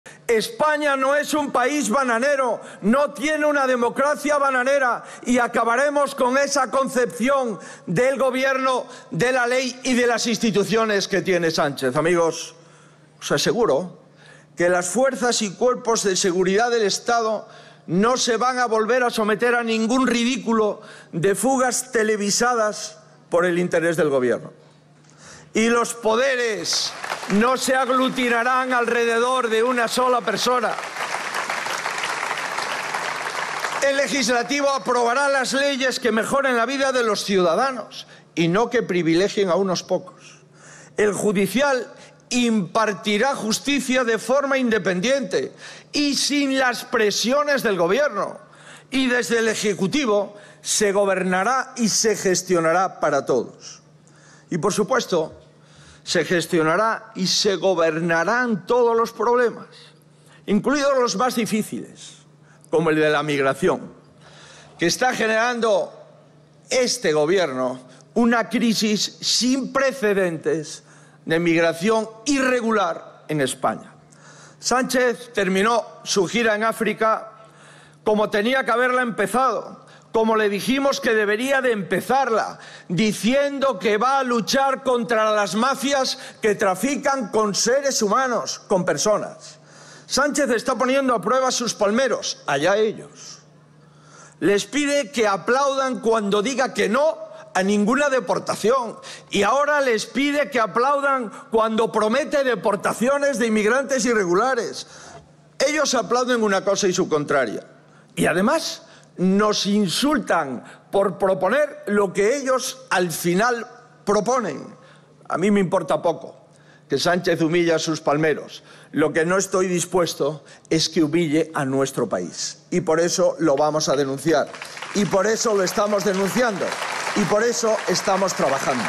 Feijóo acusa al Gobierno de generar "una crisis sin precedentes" de inmigración irregular
El presidente del Partido Popular, Alberto Núñez Feijóo, ha acusado al Gobierno de generar "una crisis sin precedentes" de inmigración irregular en España y ha asegurado que, en este nuevo curso político que ahora arranca, el PP "gestionará todos los problemas", incluida la cuestión migratoria, que ha calificado como "problema difícil".

(Fuente: PP)